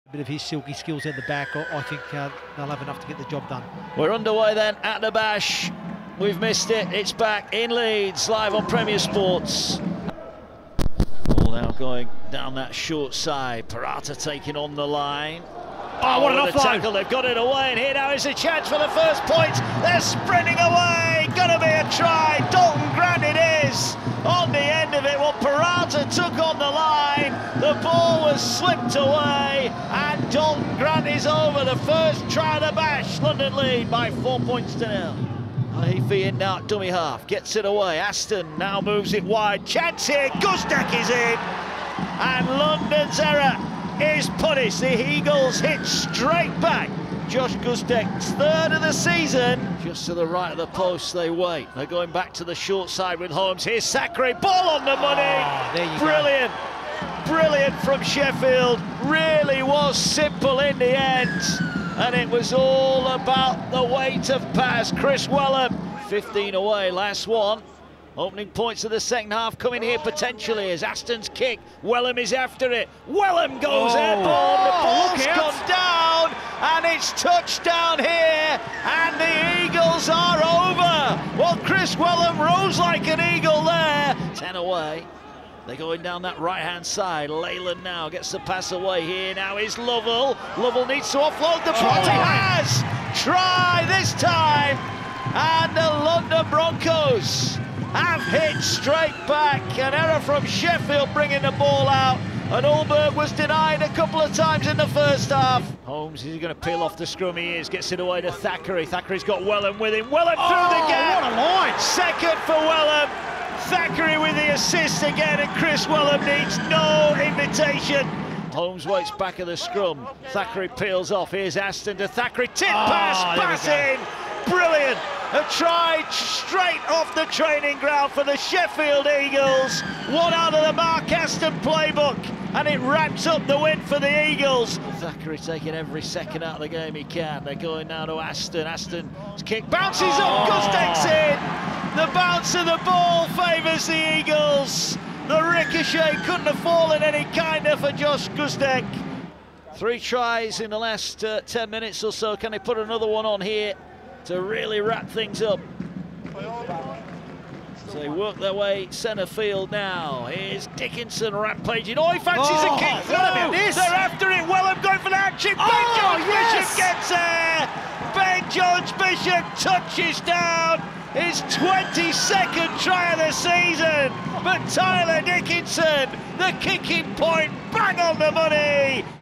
Highlights | London Broncos v Sheffield Eagles
Watch all the highlights as London Broncos took on the Sheffield Eagles in game one of the Summer Bash.